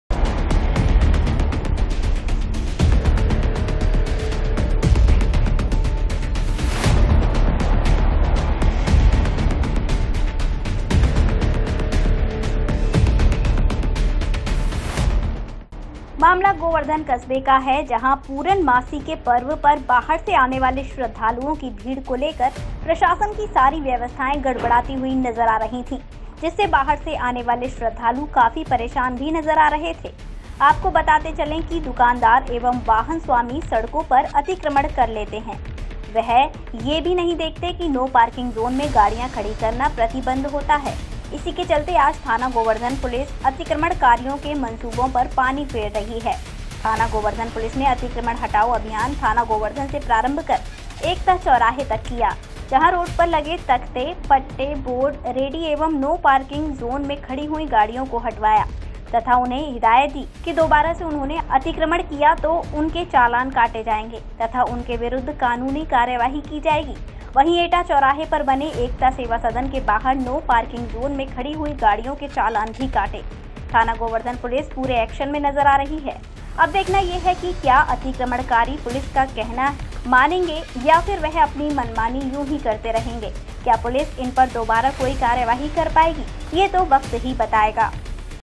गोवर्धन पुलिस हुई सतर्क, डग्गेमार वाहनों के काटे चालान
GnineNews-गोवर्धन पुलिस हुई सतर्क, डग्गेमार वाहनों के काटे चालान